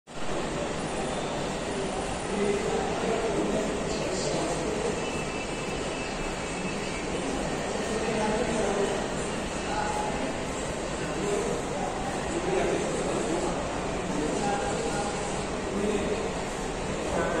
Indian Freedom Fighters || Azadi ka amrit mahotsaav || Barakhamba Road Metro Station || Delhi Metro
This poster was on display in Barakhamba Road Metro station in  the honor of freedom fighters.